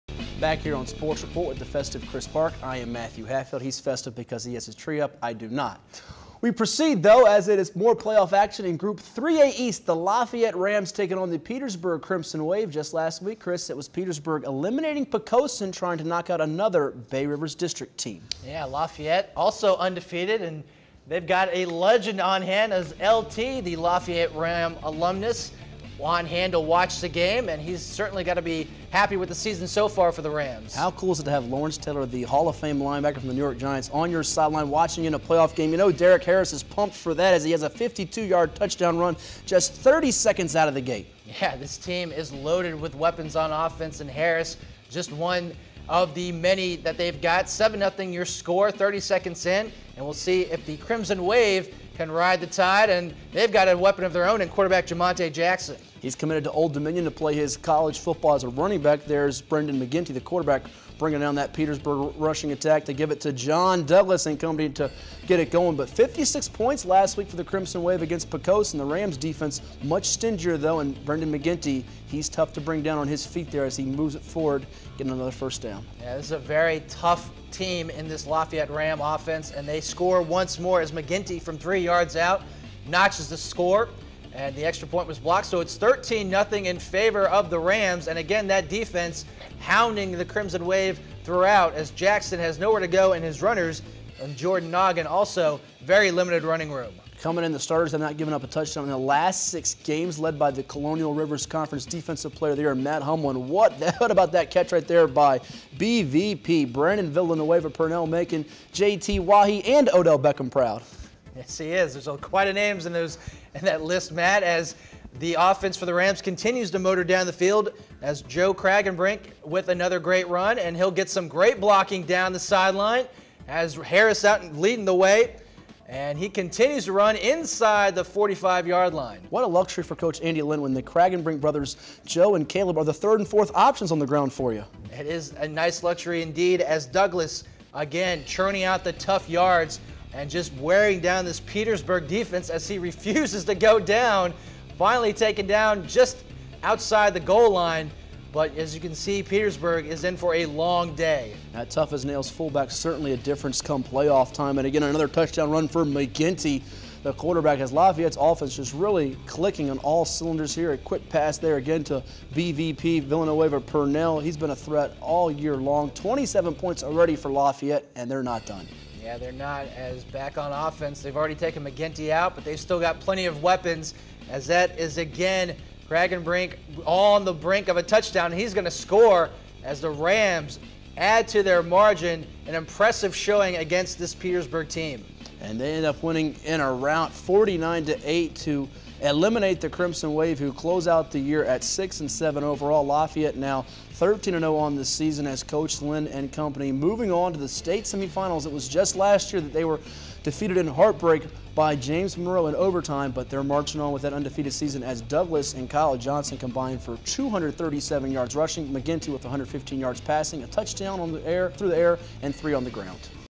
Sports Report: Petersburg @ Lafayette
Highlights from the November 28th game.